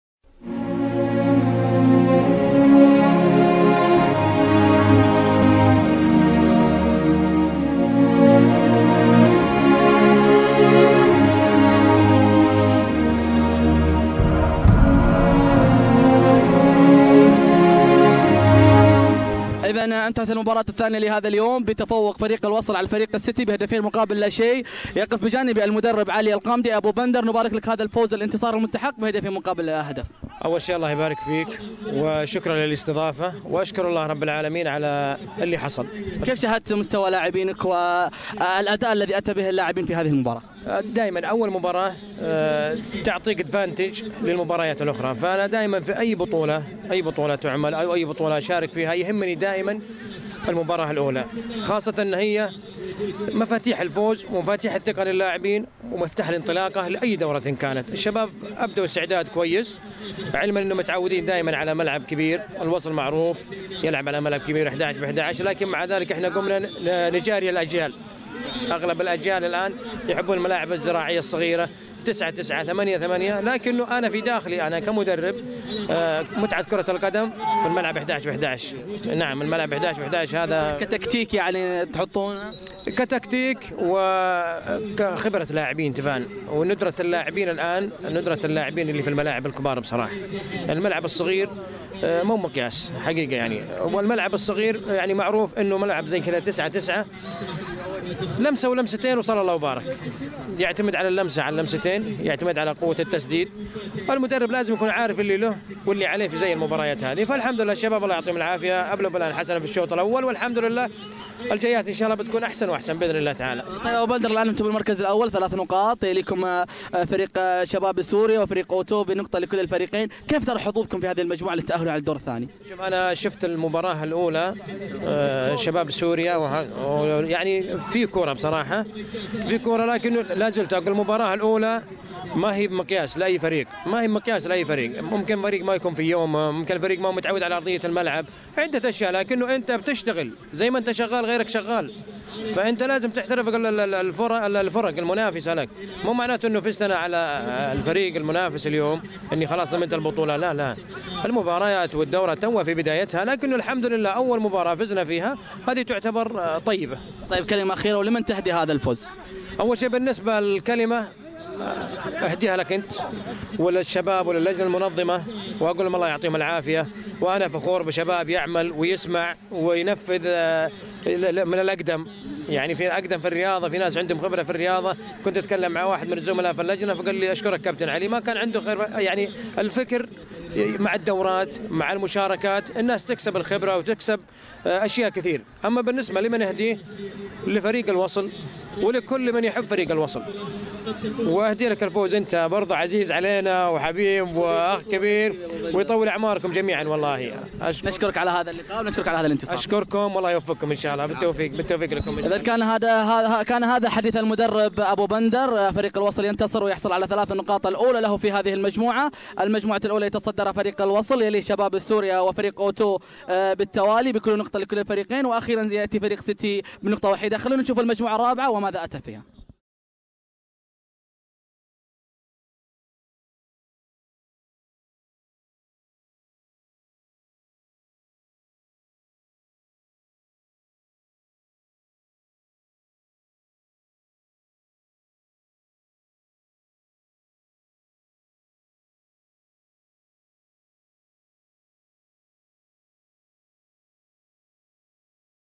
مدرب الوصل علي الغامدي بعد الفوز على موبايلي
تغطية شبكة الفارس العالمي الإعلامية والاعلانية الشريك الاعلامي الرسمي للاحياء
الموقع الرسمي